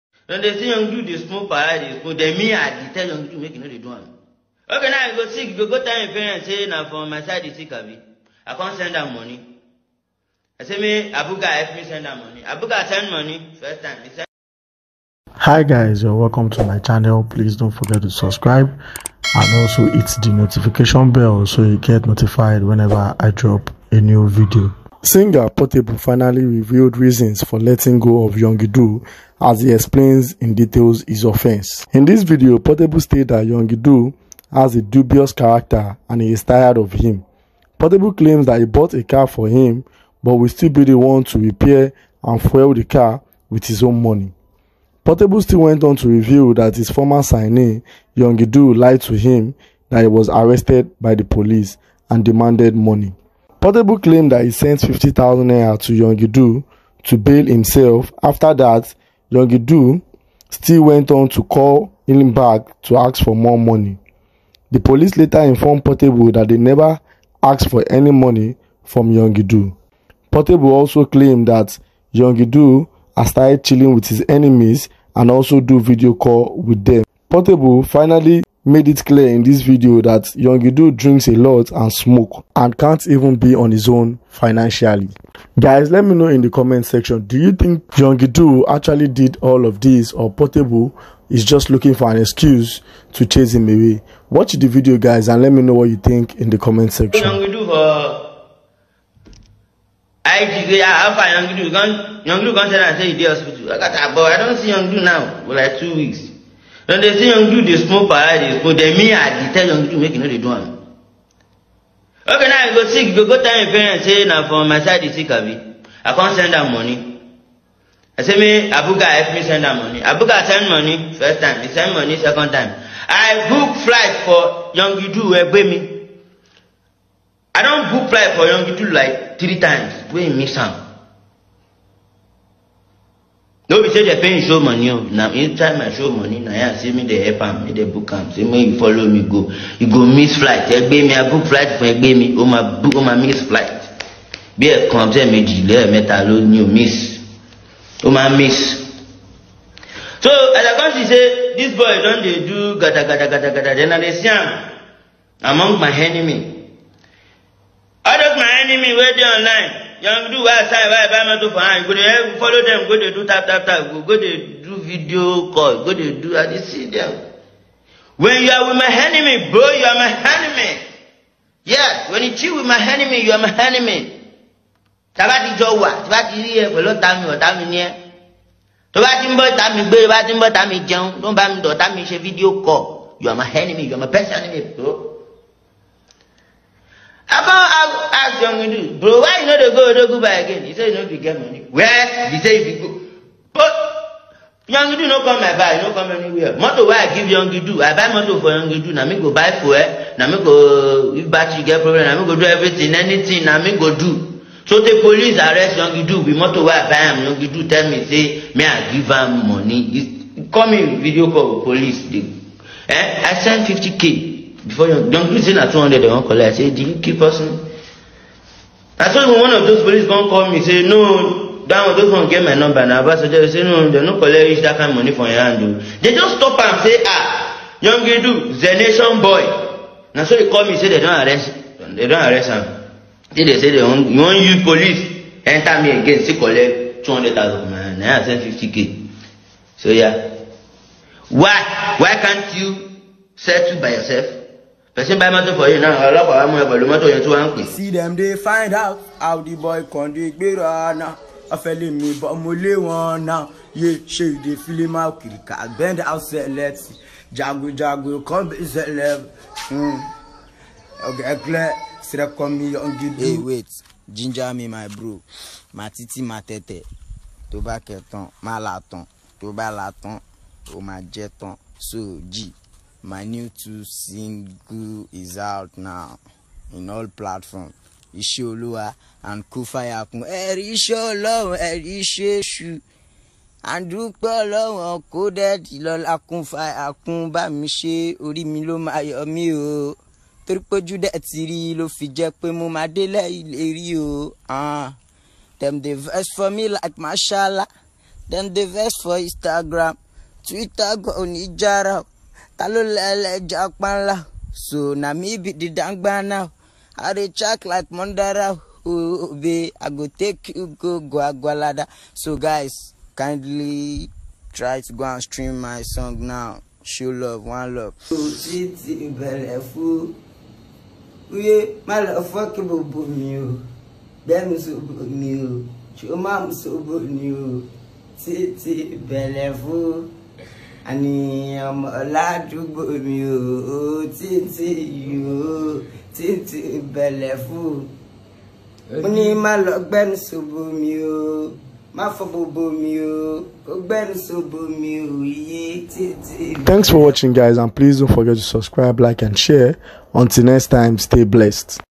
Don't Miss Out: Portable Exposes Young Duu Contract Offence
Singer Portable has Finally Revealed reasons for letting go of his Signee "Young Duu" and also gave detail of his offense